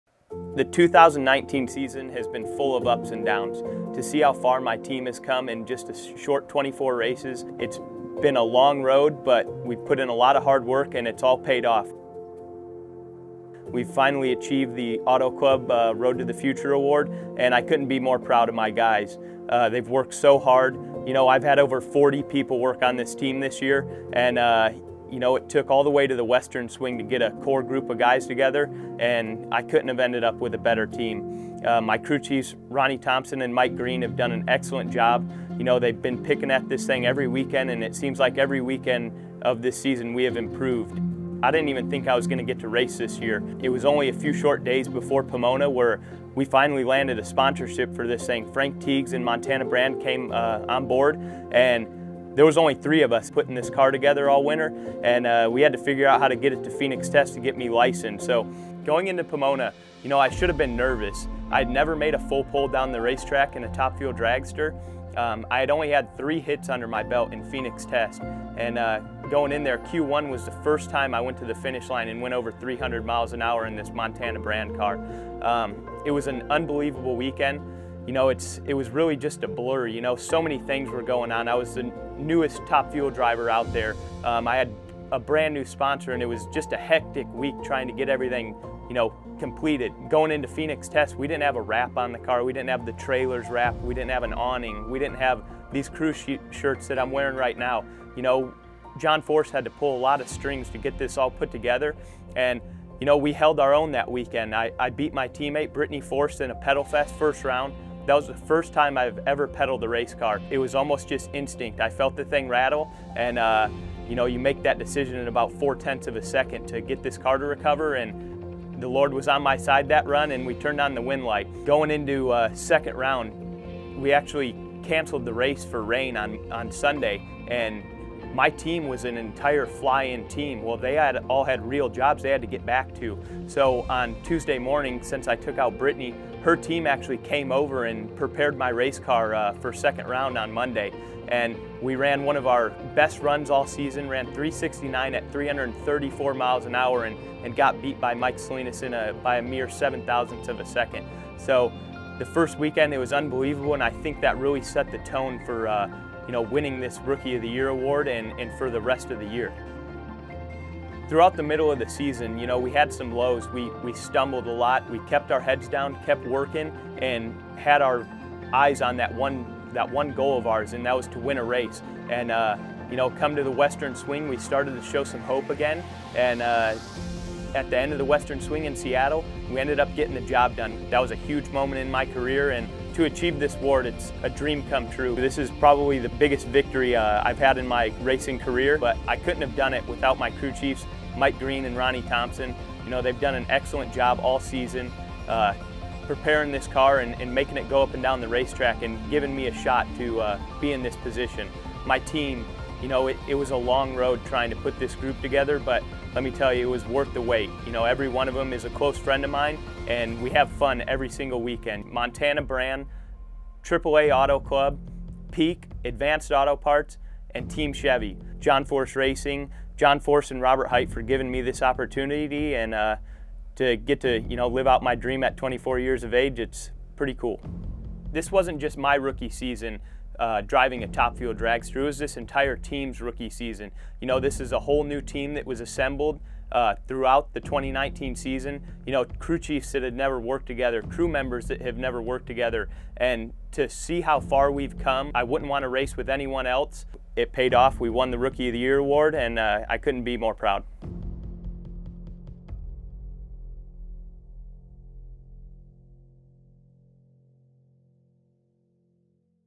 2019 Rookie of the Year Winner Austin Prock
Austin Prock is the 2019 Auto Club Road to the Future Award winner and 2019 NHRA Rookie of the Year! Here are some highlights and thoughts on his exciting rookie season driving the Montana Brand / Rocky Mountain Twist Top Fuel dragster.